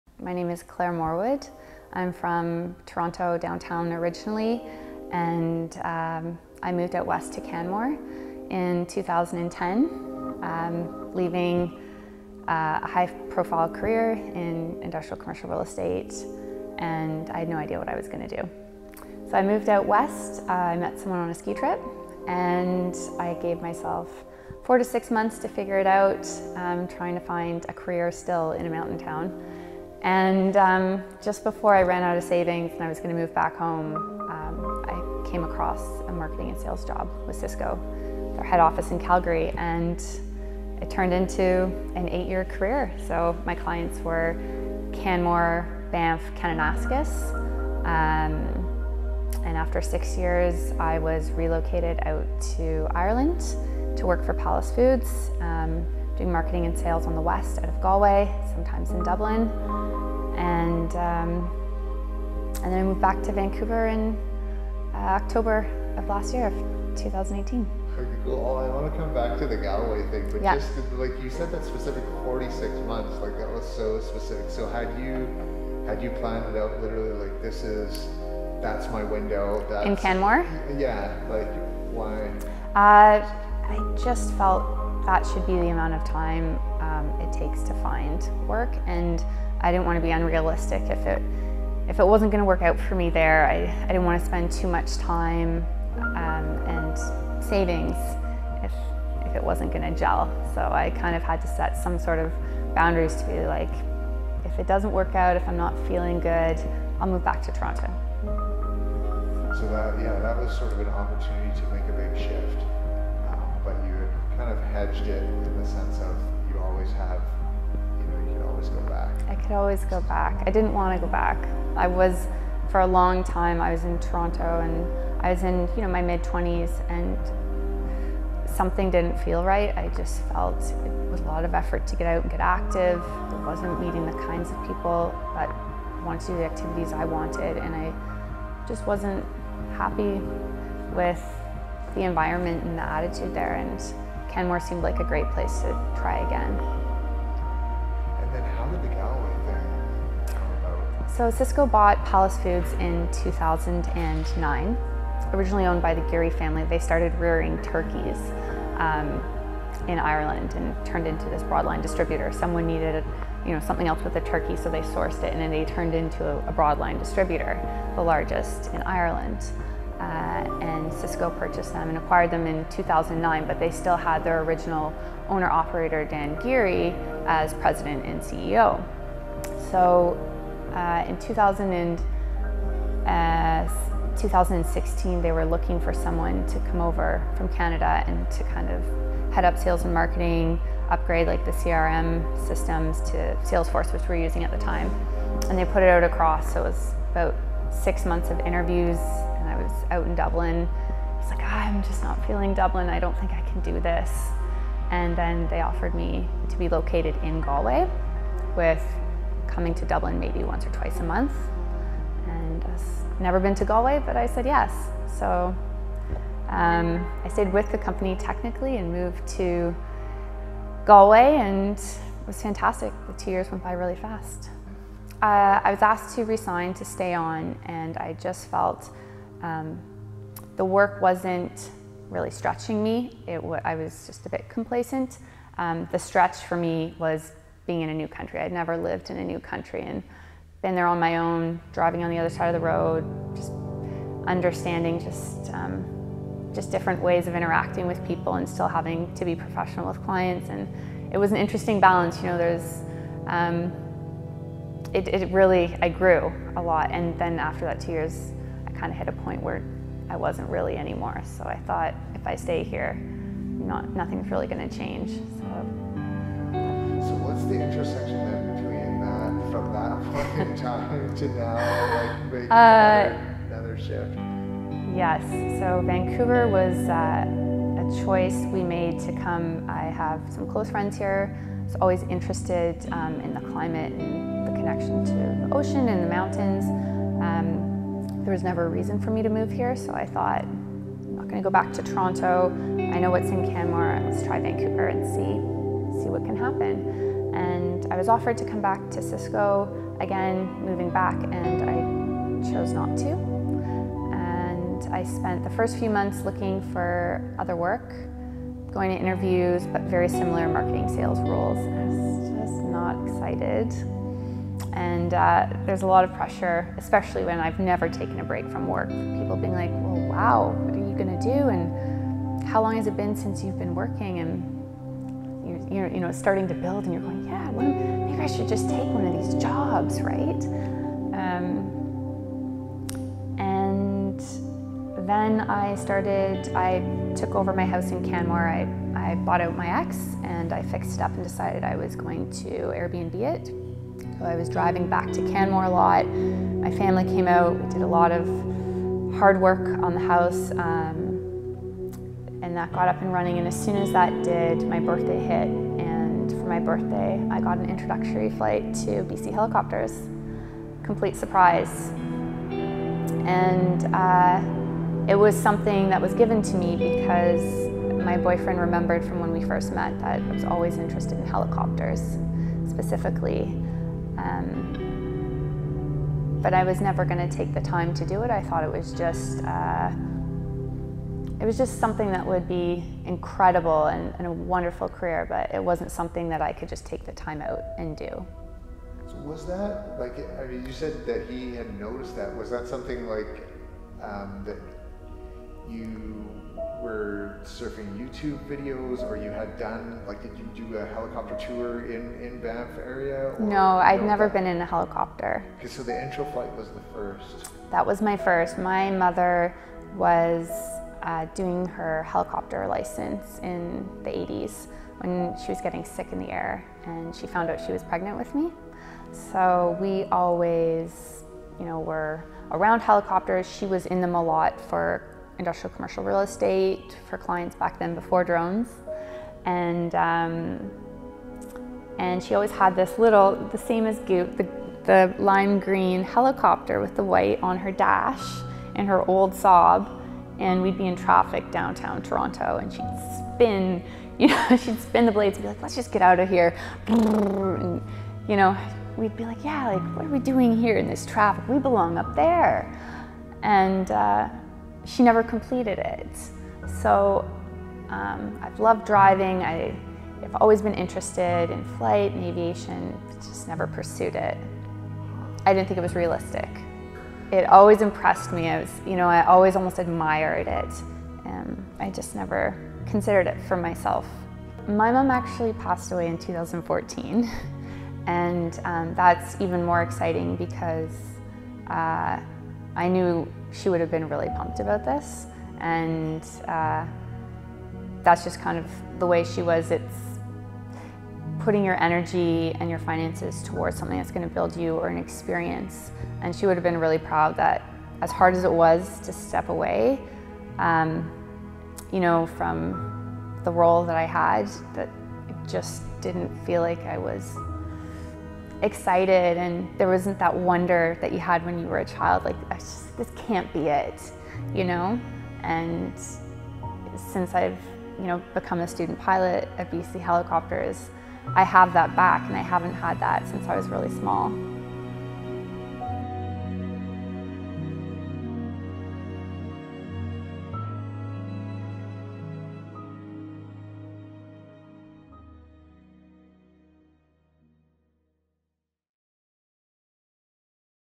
Helicopter Student Interview | Claire Morwood
This is our student. Her story is pretty remarkable. If you would like to know more about our flight school and the training we offer at BC Helicopters you can visit: